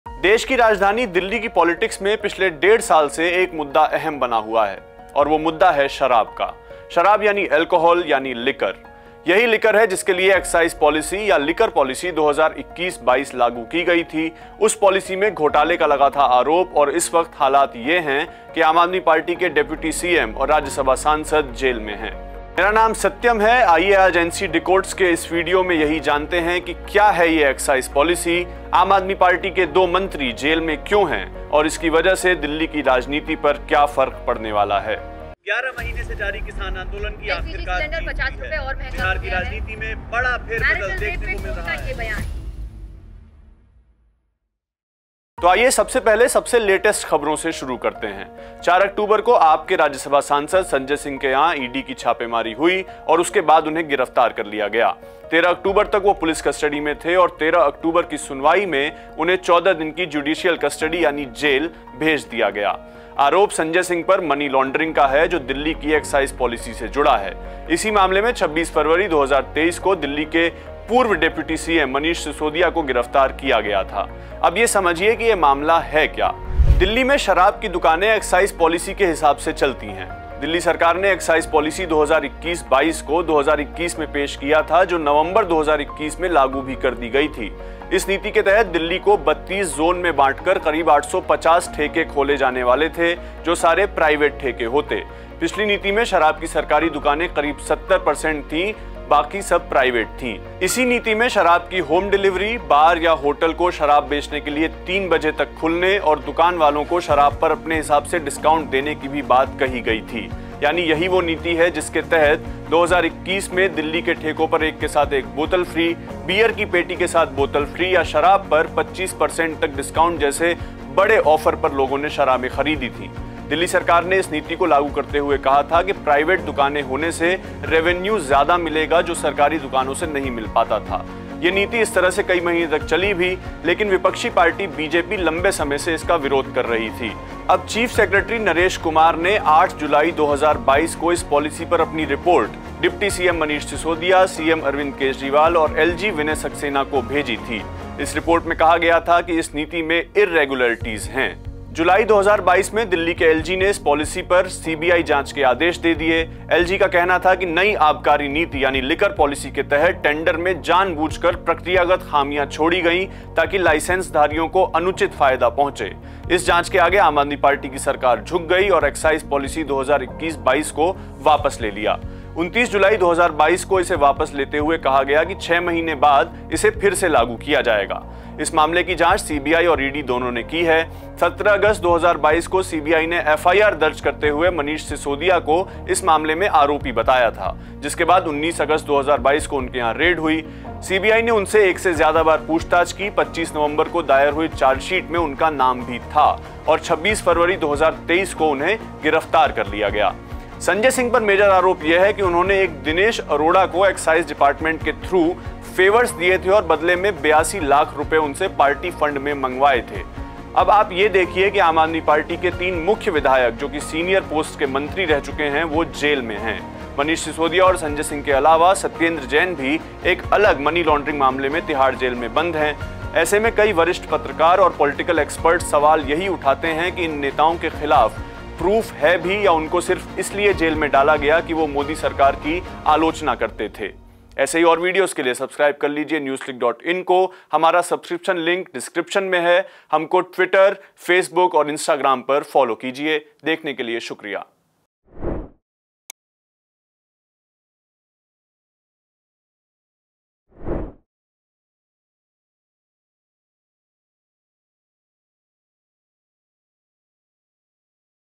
Liquor Policy Delhi : शराब ‘घोटाले' में अब तक क्या-क्या हुआ?
Delhi Excise Policy 202 -22 में घोटाले के आरोप आम आदमी पार्टी की सरकार पर लगे हैं। जिसके तहत अब तक पार्टी के नेता और दिल्ली के पूर्व deputy cm Manish Sisodia और राज्यसभा सांसद Sanjay Singh जेल में हैं। NCDecodes के इस वीडियो में जानिये कि यह शराब नीति क्या है, आप नेताओं पर आरोप क्या हैं और इसकी वजह से दिल्ली की राजनीति पर क्या असर पड़ रहा है?